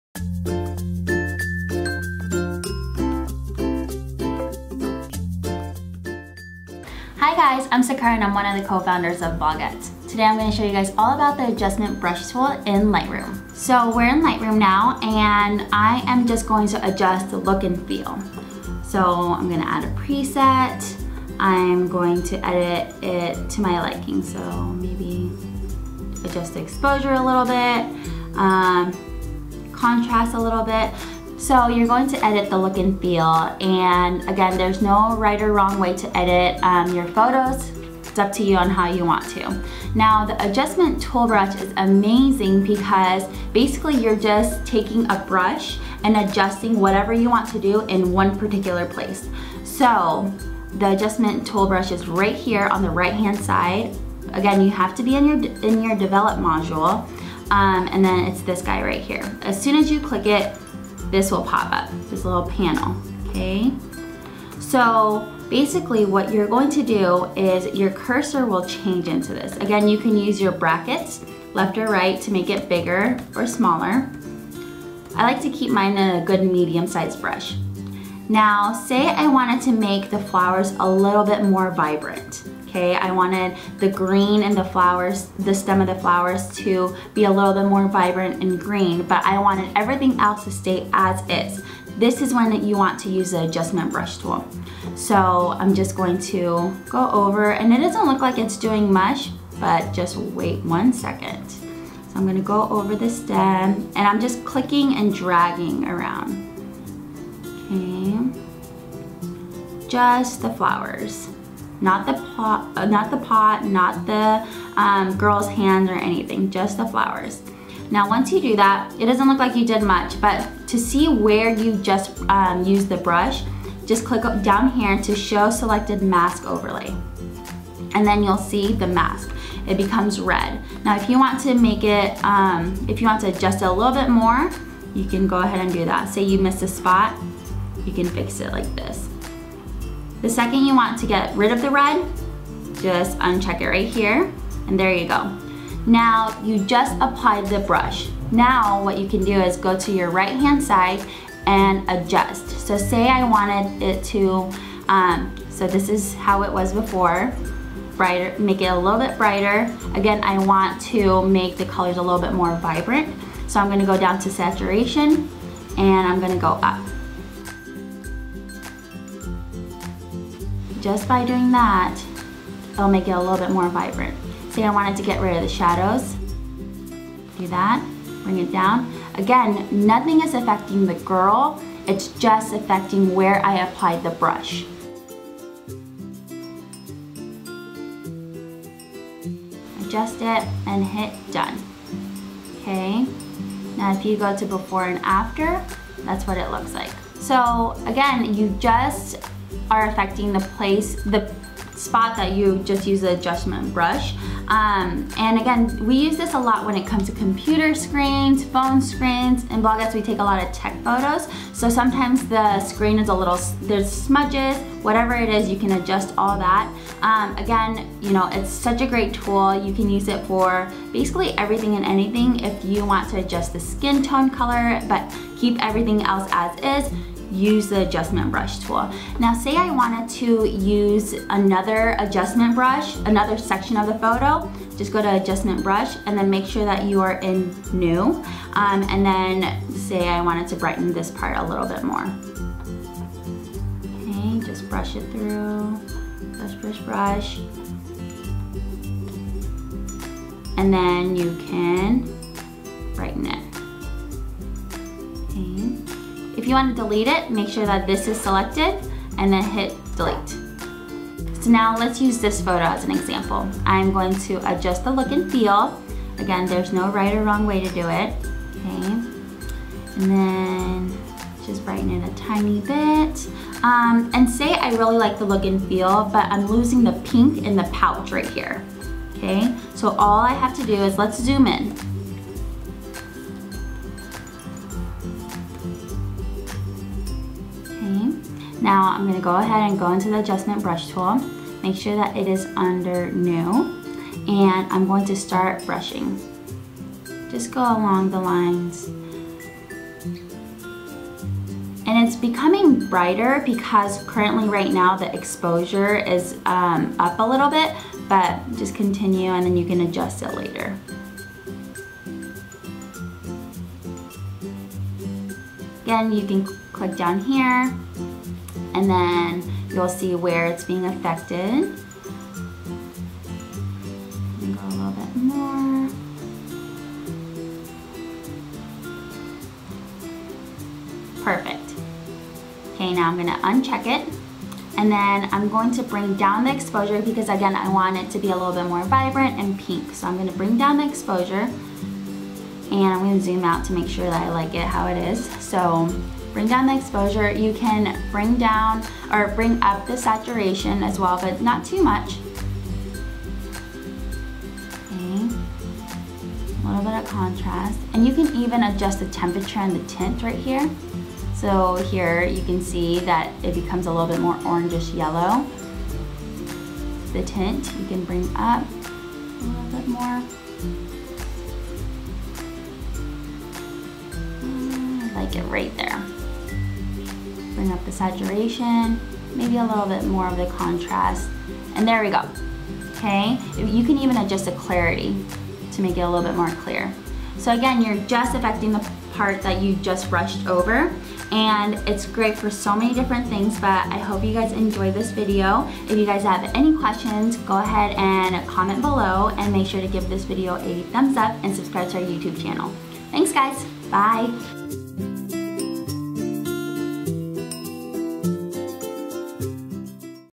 How To: Use The Brush Tool In Lightroom
At Bloguettes, we edit almost every one of our photos in Lightroom, so it's definitely a go to for us and we've developed some experience throughout the years with the software. Every once in a while we need a little bit more a touch up on a photo than just a filter. Today we are here to show you how to use the brush tool in Lightroom. It's important to learn the tools in Lightroom because not only is it easier to multitask an edit and a filter, it's also more widely known than learning all the separate tools in photoshop. Using photoshop instead? Head over to our other videos to check out how to use the brush tool in photoshop too! Don't forget to thumbs up this video so we know to make more! Until next time!